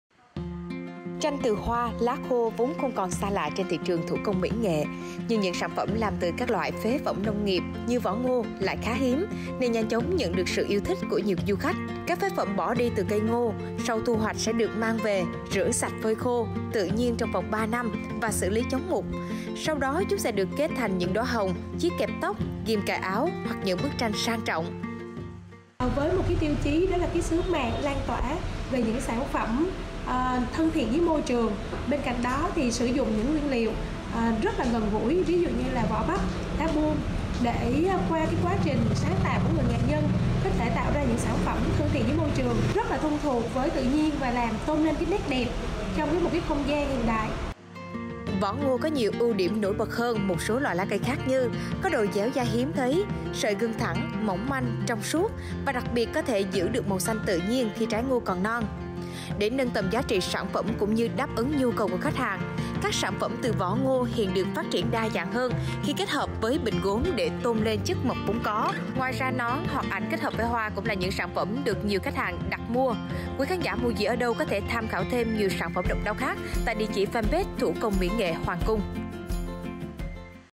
Hô biến vỏ bắp ngô thành kẹp tóc, bình hoa xinh xắn
Các phế phẩm bỏ đi từ cây ngô sau thu hoạch sẽ được mang về, rửa sạch, phơi khô tự nhiên trong 3 năm và xử lý chống mục... Sau đó, chúng sẽ được kết thành những đóa hoa hồng, chiếc kẹp tóc, ghim cài áo hoặc những bức tranh sang trọng…